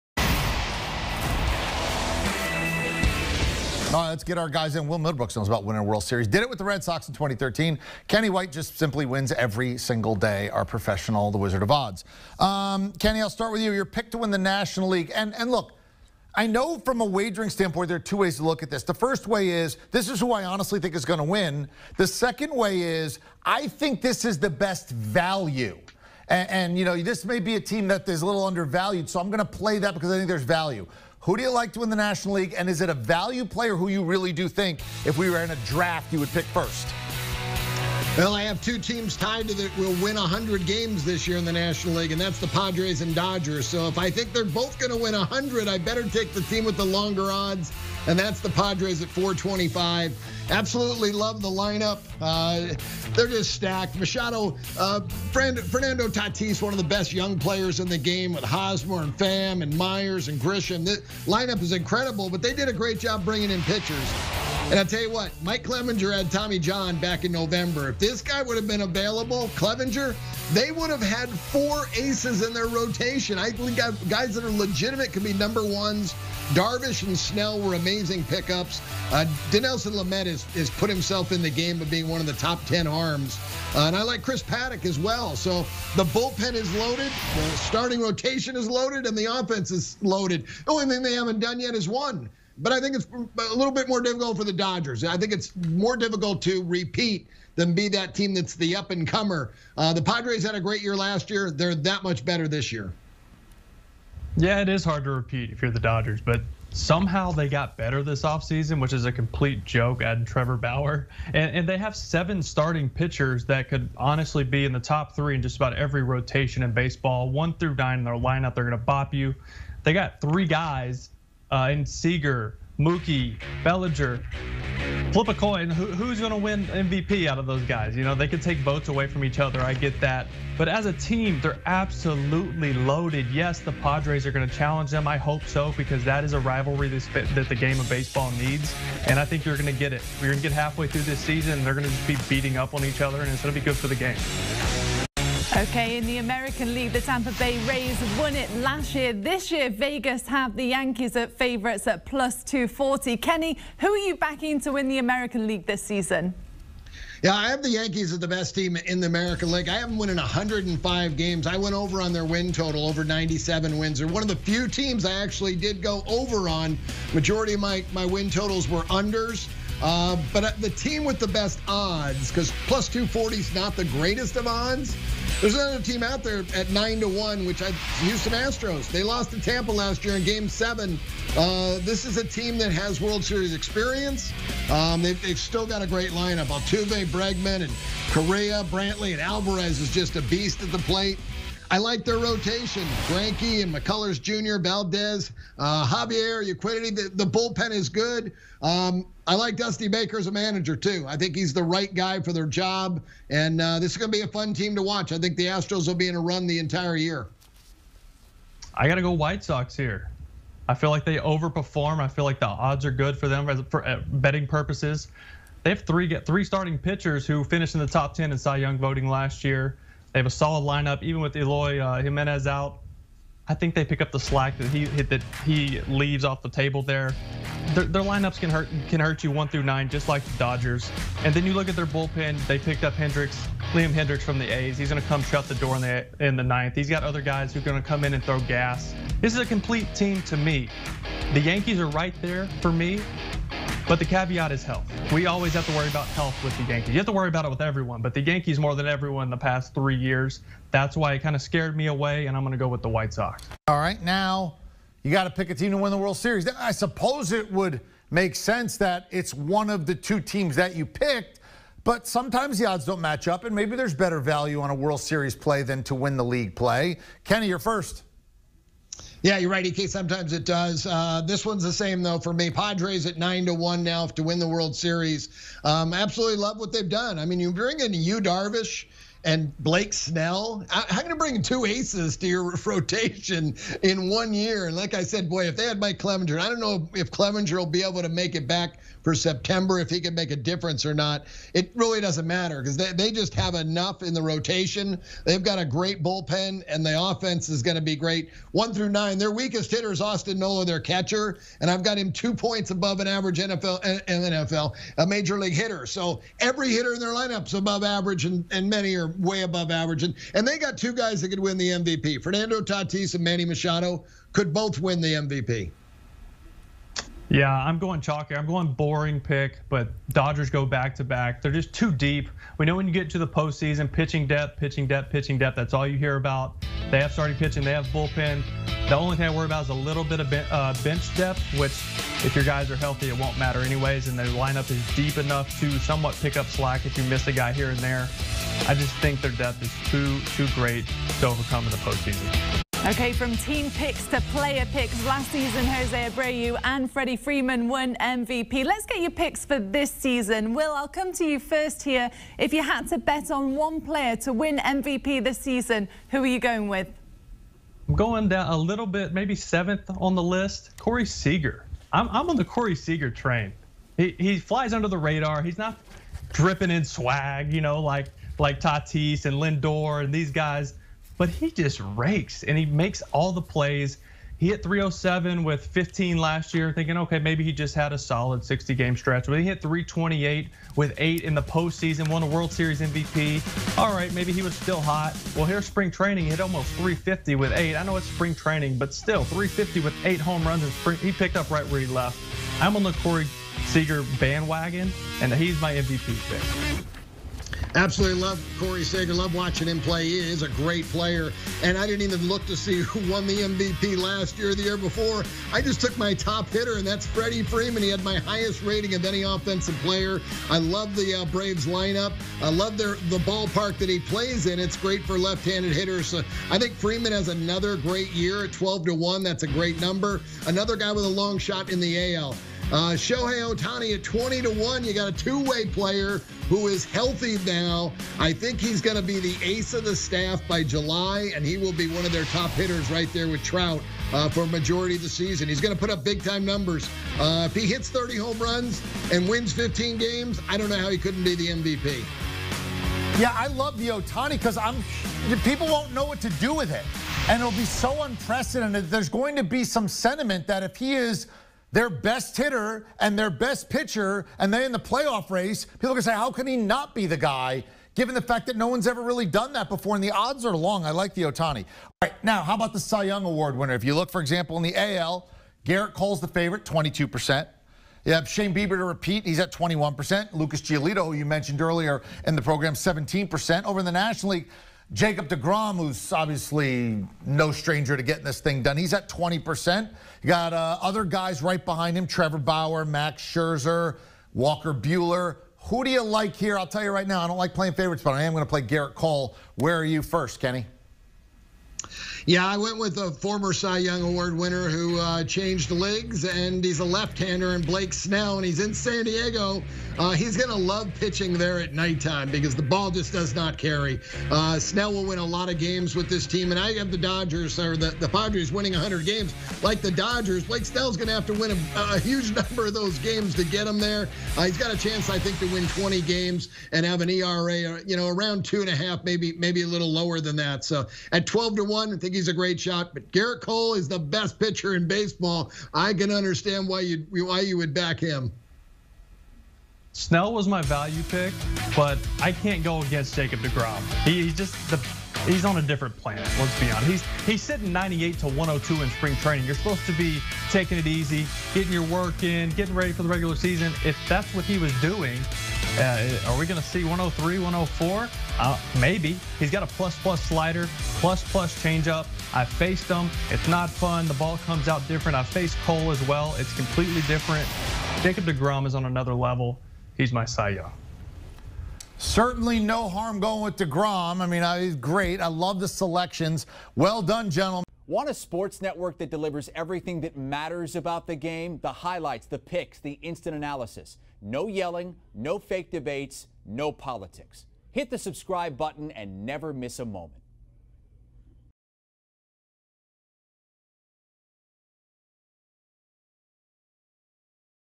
MLB: Picks to Win National League, American League, & World Series | CBS Sports HQ
Will Middlebrooks and Kenny White join CBS Sports HQ to give their picks to win!